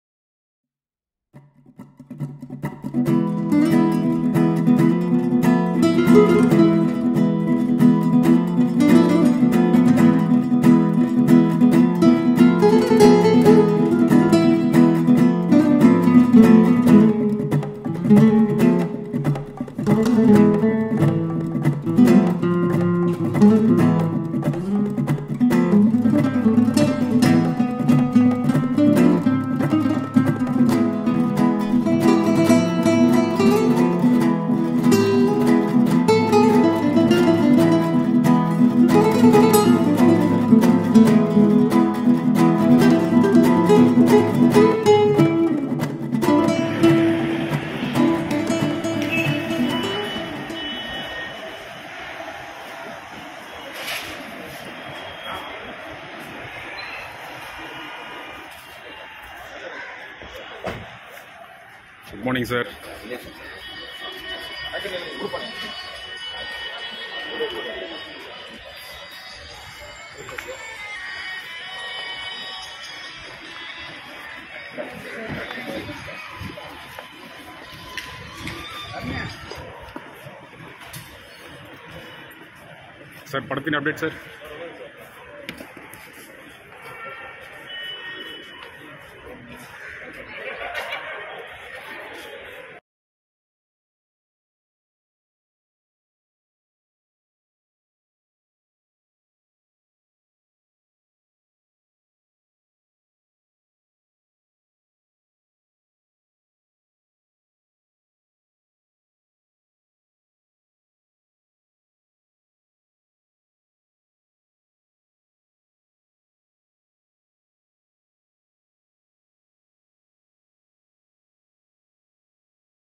Vettaiyan படப்பிடிப்பில் பங்கேற்பதற்காக ஹைதராபாத் புறப்பட்டார் Rajinikanth!
Rajinikanth Vettaiyan Shooting | வேட்டையின் படப்பிடிப்பானது மூன்றாம் கட்ட படப்பிடிப்பில் பங்கேற்பதற்காக நடிகர் ரஜினிகாந்த் ஐதராபாத் செல்வதற்கு சென்னை விமான நிலையம் வந்தார் சென்னை விமான நிலையத்தில் அவரது ரசிகர்களுடன் இன்று புகைப்படங்கள் எடுத்துக் கொண்ட பின்பு விமான நிலையத்திற்கு உள்ளே புறப்பட்டு சென்றதும் குறிப்பிடத்தக்கது.